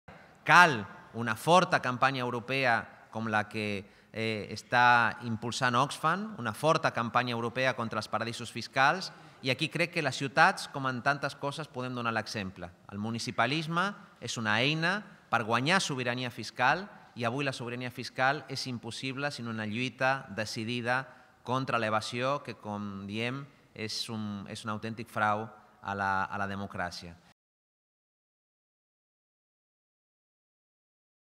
Gerardo Pisarello: "El municipalisme és una eina per guanyar sobirania fiscal".
El tinent d'alcaldia ha explicat que és necessària una forta campanya europea com la que està impulsant Oxfam contra els paradisos fiscals, i que les ciutats poden donar exemple.